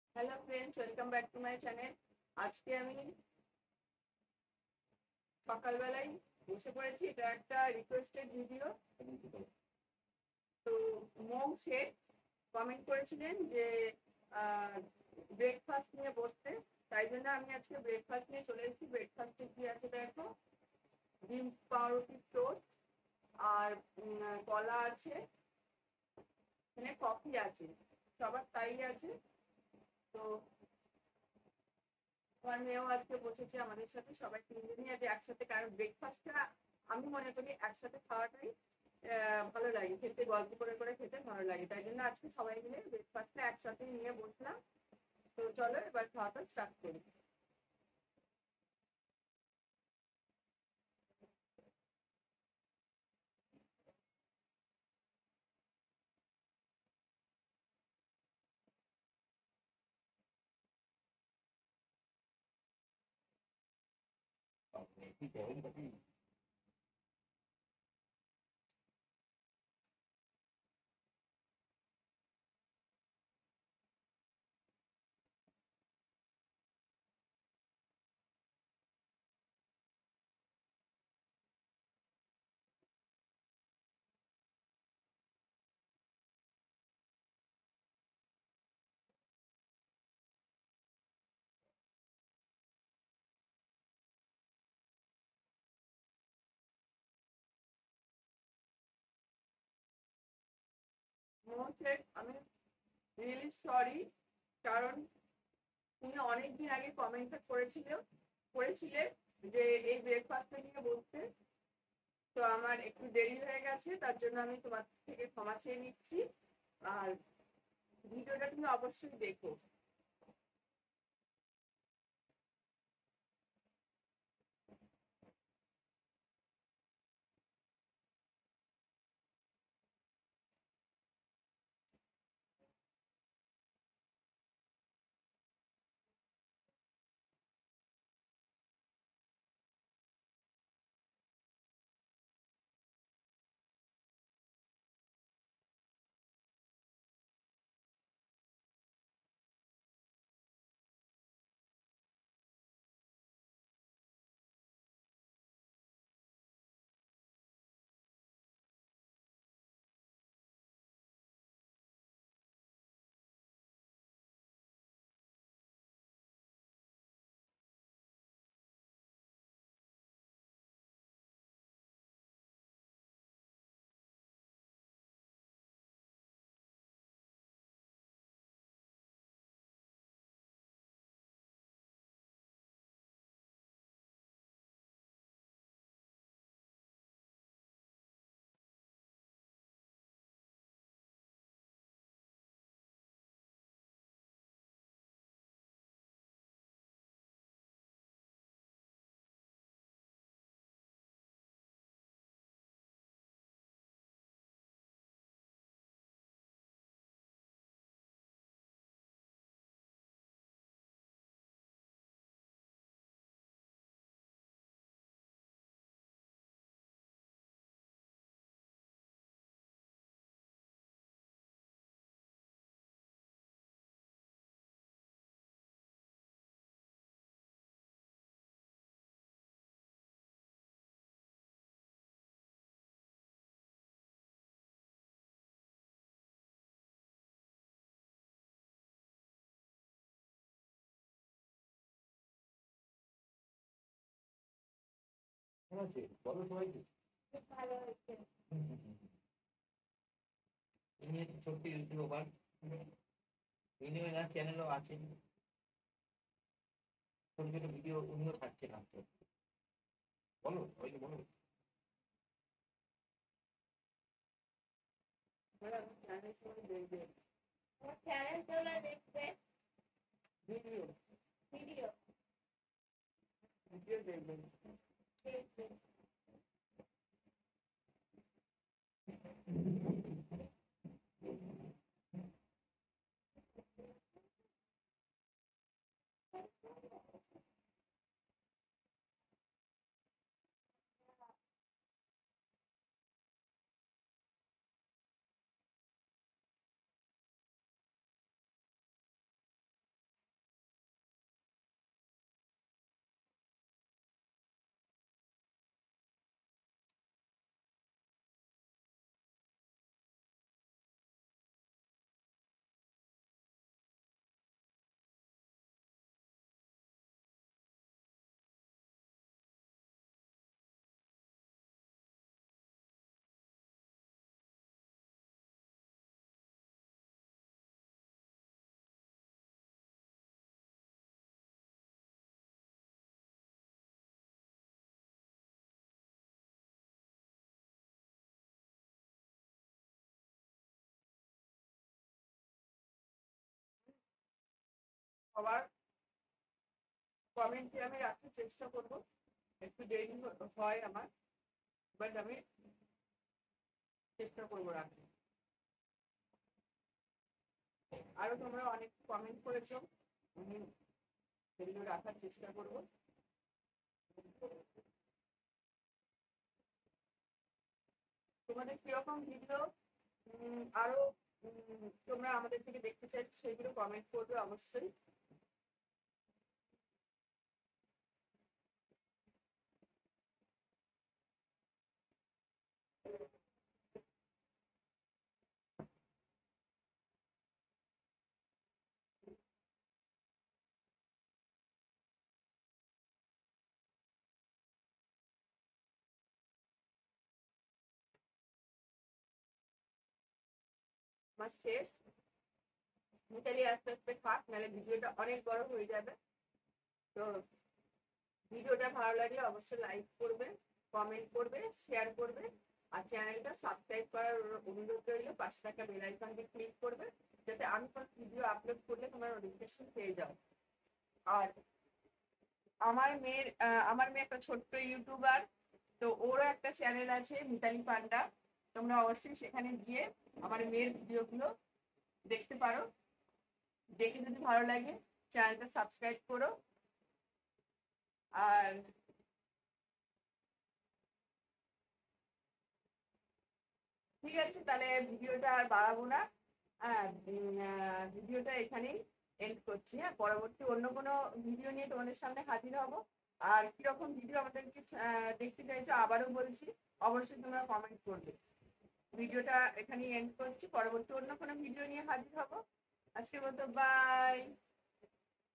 Noorie's Eating Show | Having Breakfast with Family... Bread Omlet, Banana, Coffee...
No. 84

My Older Videos